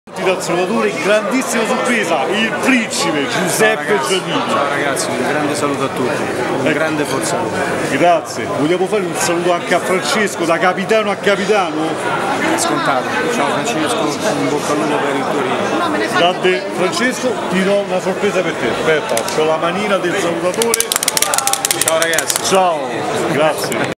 Capitan Giannini saluta i tifosi della A.S. Roma e Totti col Salutatore
Capitan Giuseppe Giannini saluta Francesco Totti e i fans con il Salutatore